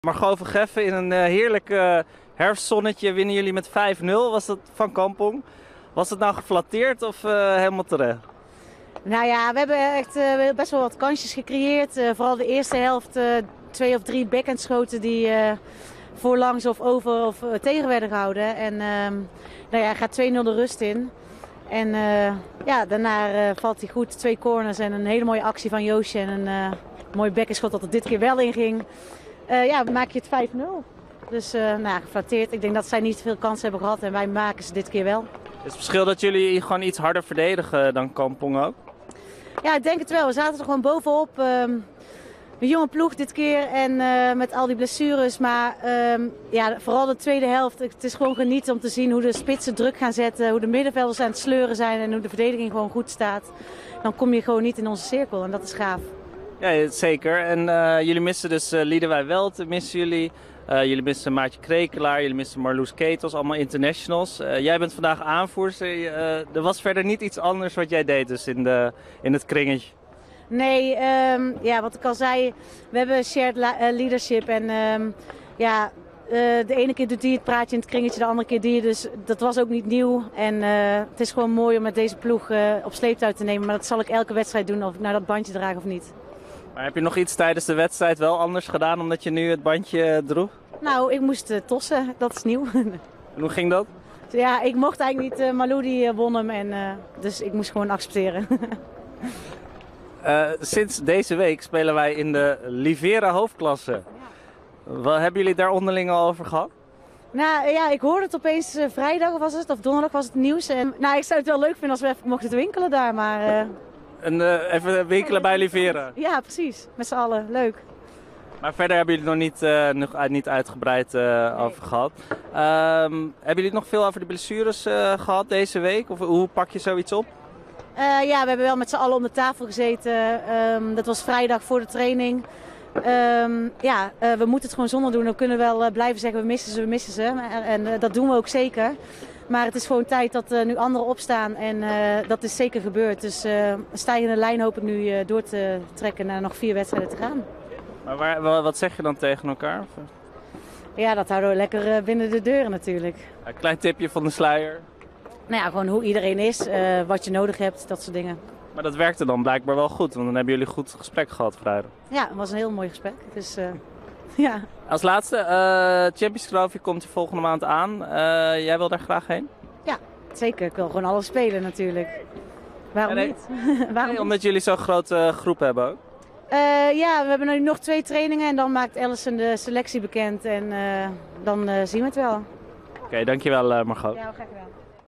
Margot van Geffen: 'Mooi om deze ploeg op sleeptouw te nemen'
Margot van Geffen als nieuwe captain van Den Bosch over de wedstrijd tegen Kampong. Dat ondanks blessures van Lidewij Welten, Marloes Keetels en Maartje Krekelaar met 5-0 won.